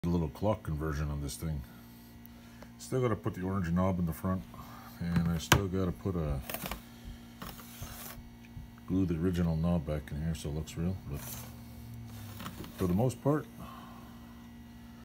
Fisher Price Clock Project
Working on making this old broken toy into a working clock...